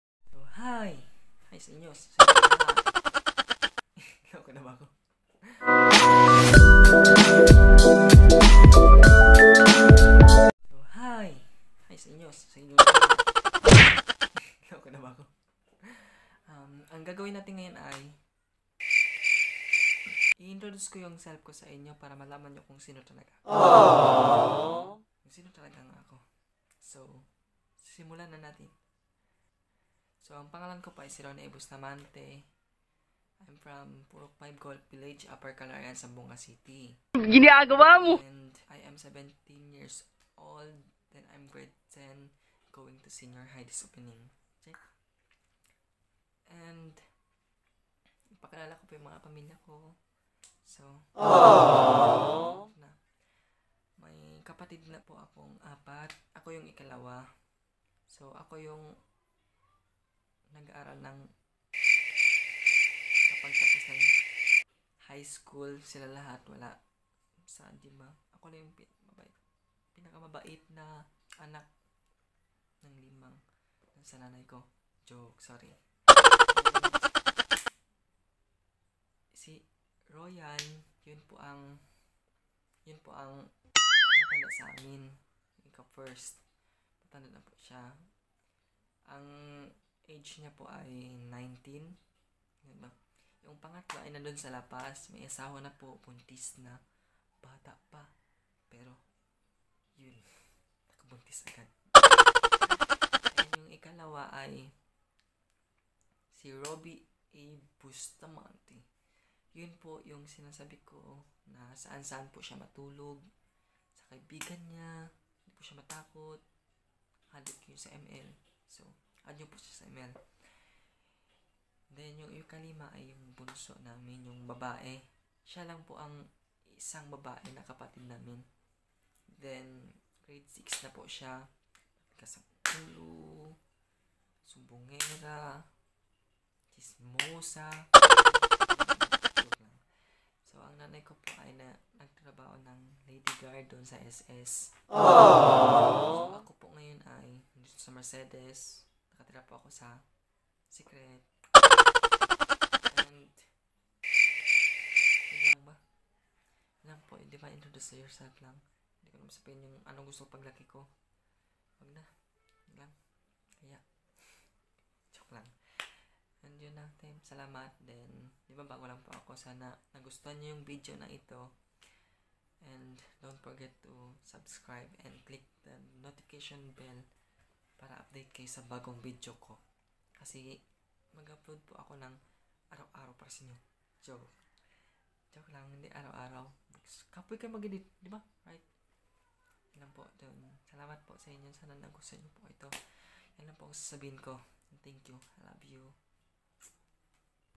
I introduce myself and my family♥️ -RONNIE BUSTAMANTE
I introduce myself and my family♥️
 This is my first ever vlog on youtube channel ko ♥️